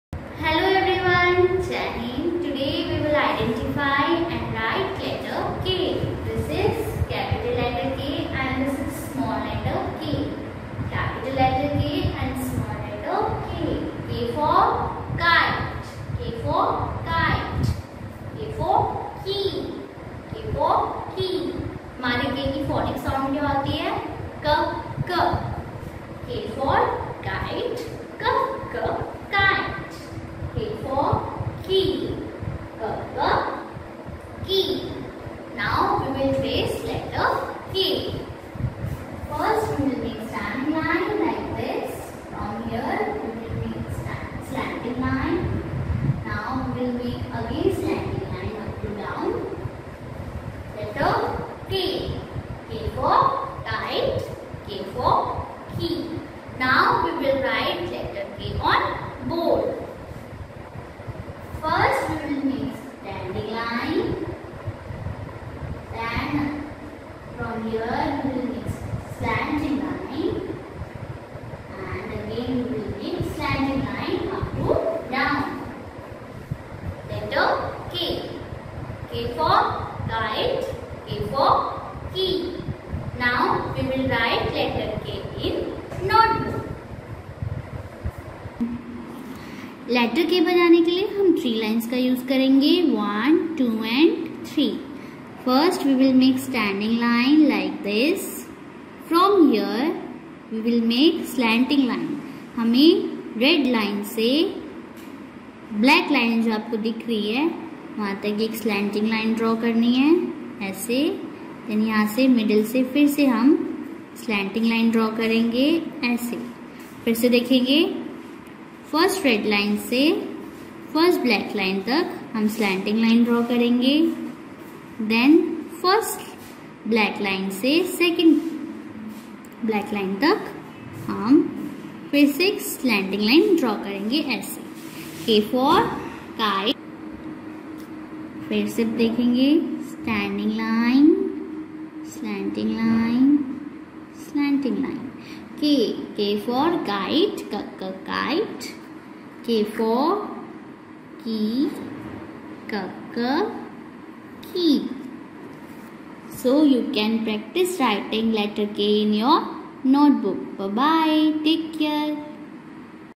Letter K | Writing Capital Letters | Alphabet Writing | Manthan School
In this video you will identify some objects that begin with letter K and also learn how to capital letter K